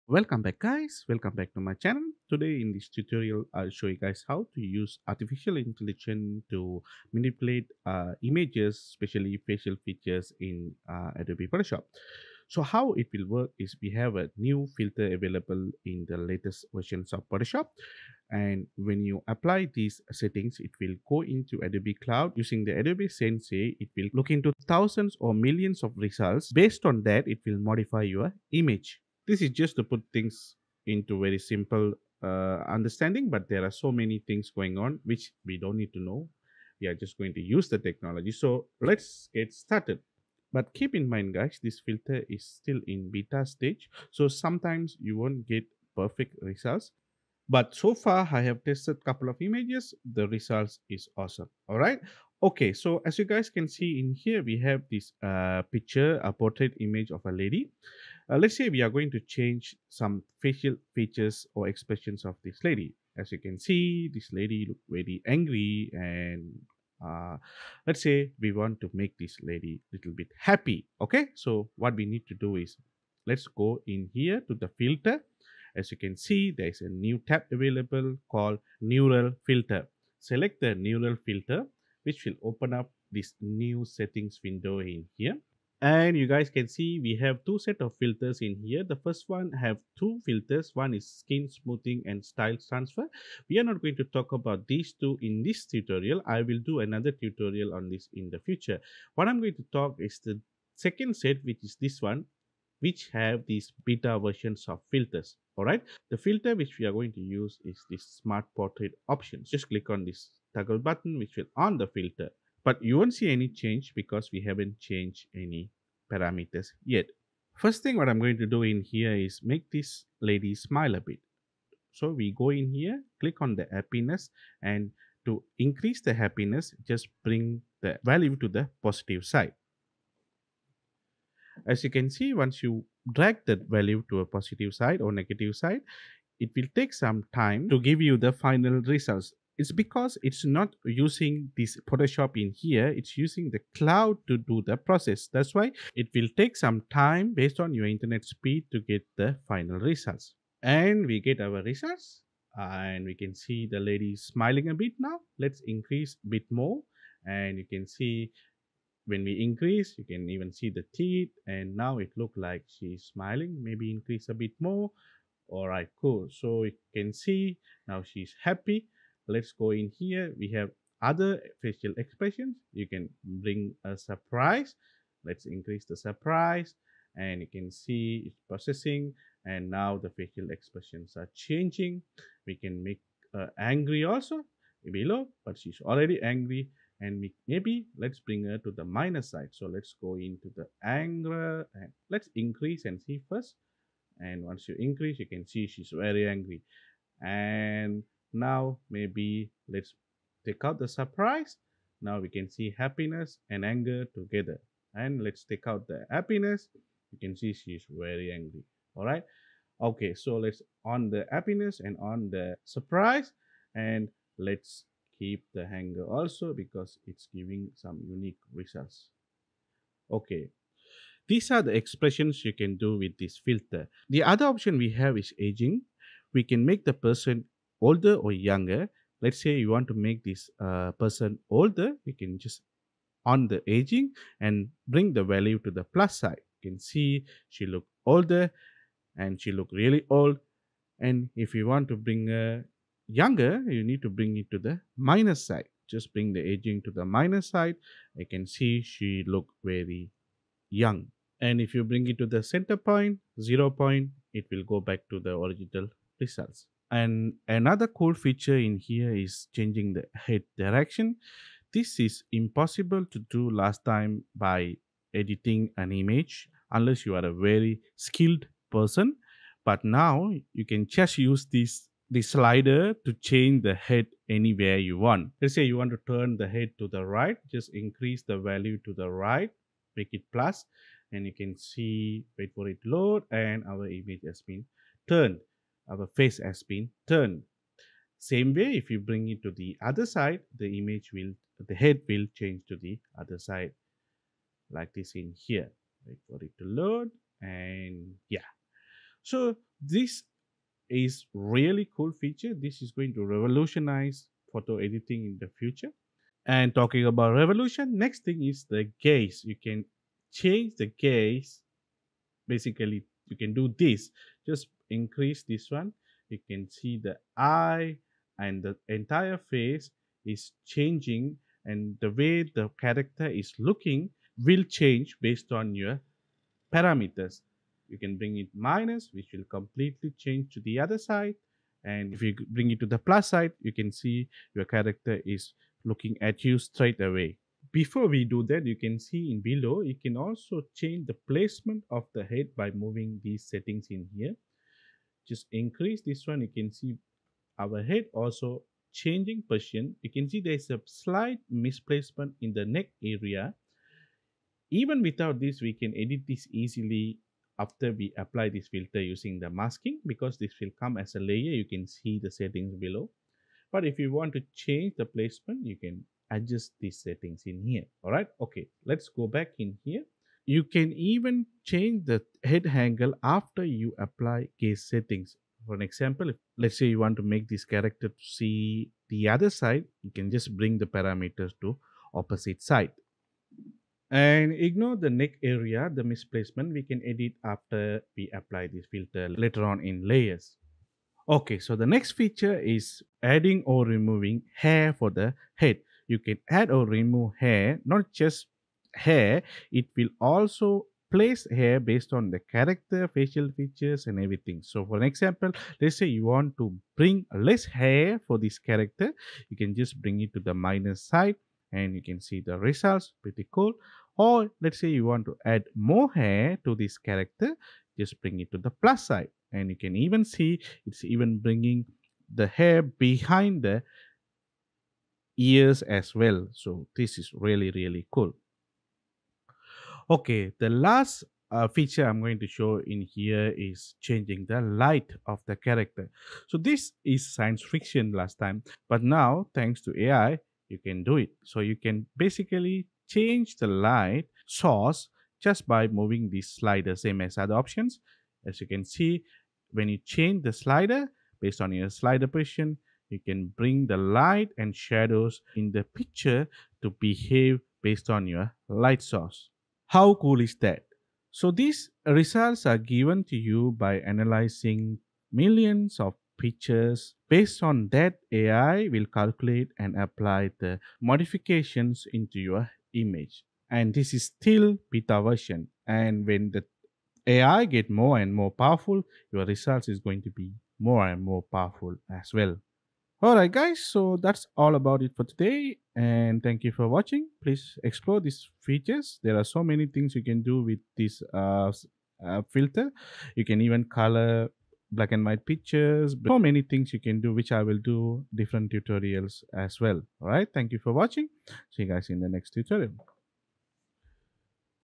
How to use AI in Photoshop to manipulate Facial Expressions | Tutorial
Hi Guys! In this tutorial I show you guys how to use artificial intelligence technology to modify facial expressions of a portrait image in photoshop.
This AI editing feature is added with neural filters in latest version of photoshop 2021. Keep in mind most of Neural filters are still in beta version. But it's getting better every day with machine learning.